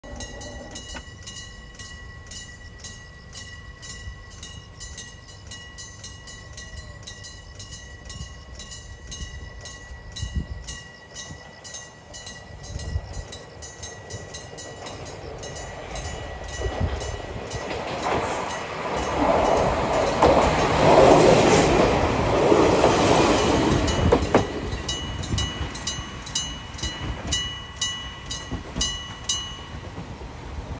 小湊鐵道上総村上⇒五井を走行するキハ40ｰ2
東北地域本社色を堅持したまま小湊鐵道にデビューした元只見線用キハ40ｰ2が今日は五井⇔光風台の運用についていました。